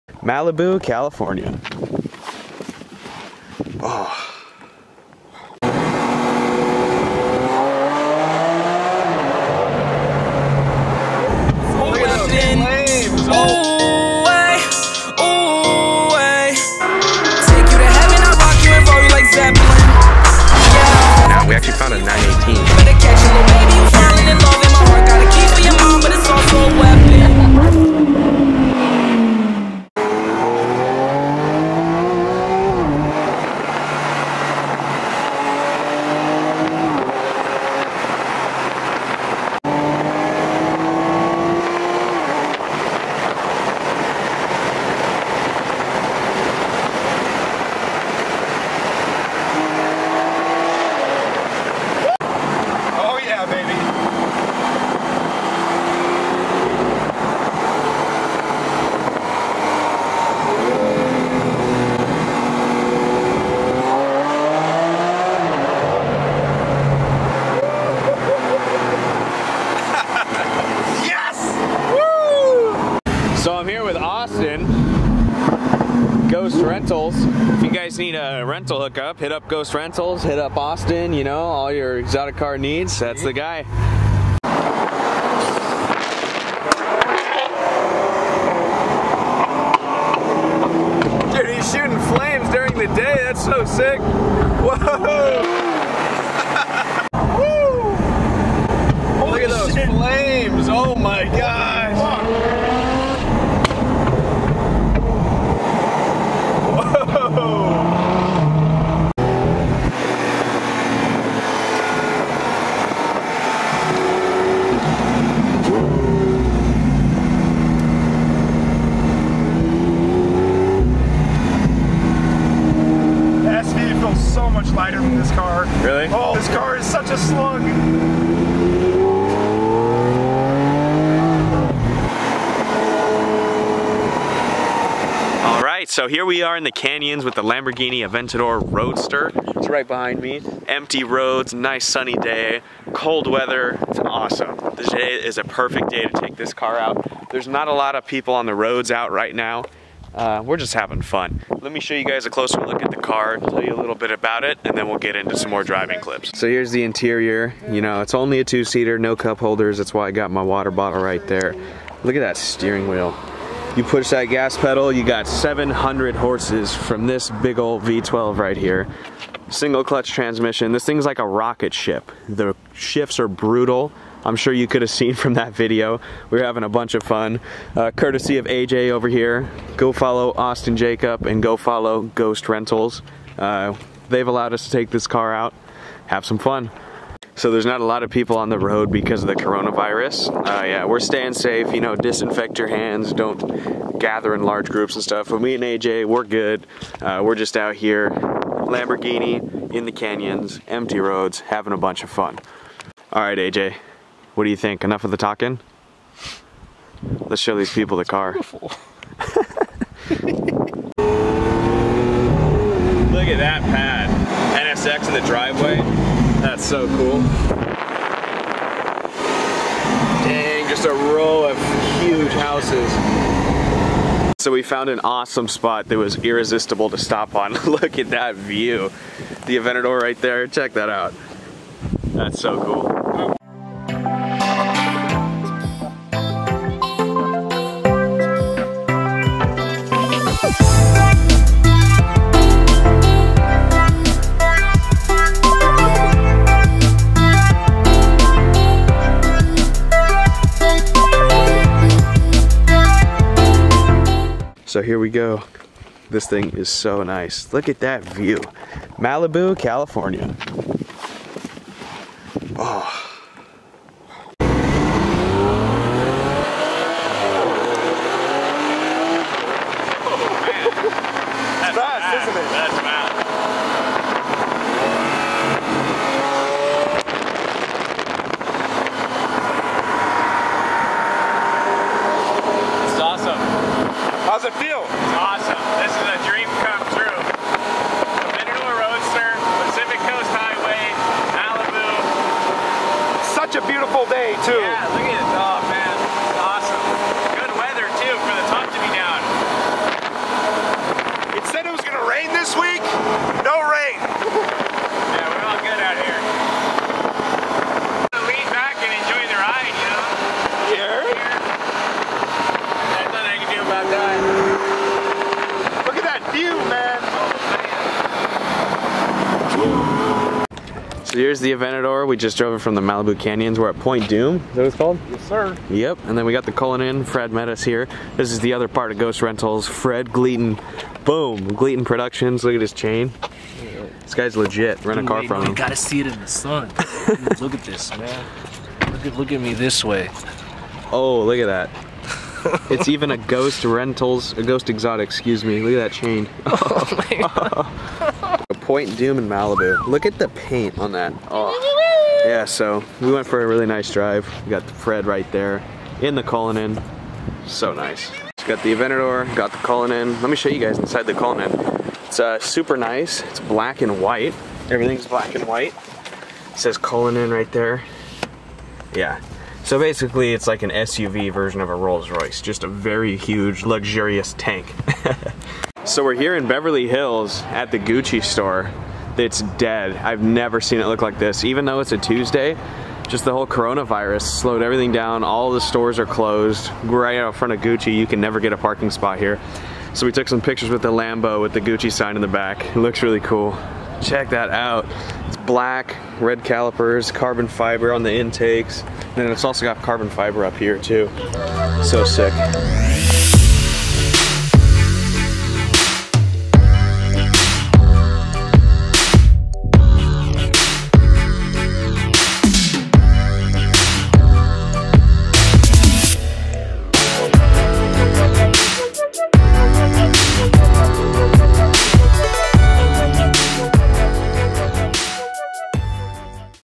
Beverly HIlls Lamborghini Aventador Roadster Shenanigans! (OPEN ROADS)
Today @GhostRentals let us have some fun in the Malibu Canyons! The roads were very open because of the Covid-19 Coronavirus!

My Gear!

SD Cards